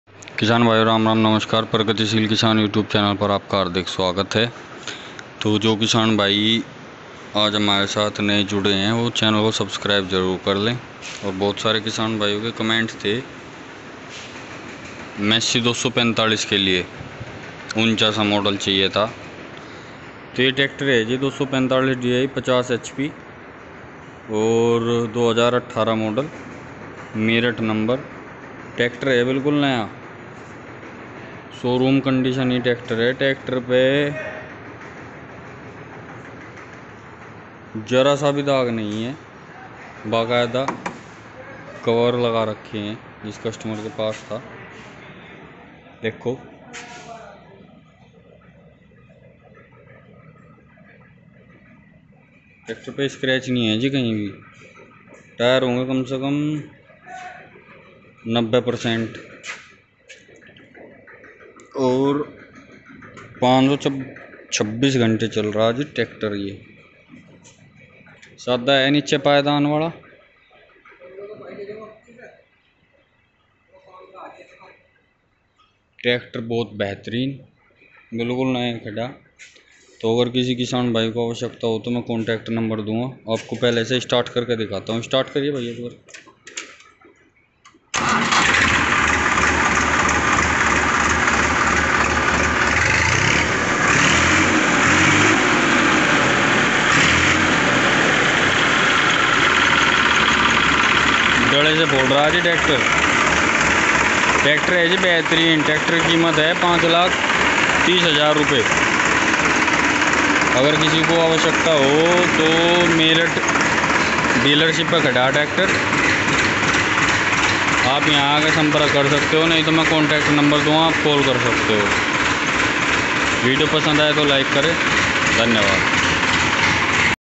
बिकाऊ Massey Ferguson 245 Di मेरठ नंबर राजधानी ट्रेक्टर मेरठ एजेंसी पर खड़ा है।
सम्पर्क करें सुबह 9 बजे से शाम 7 बजे तक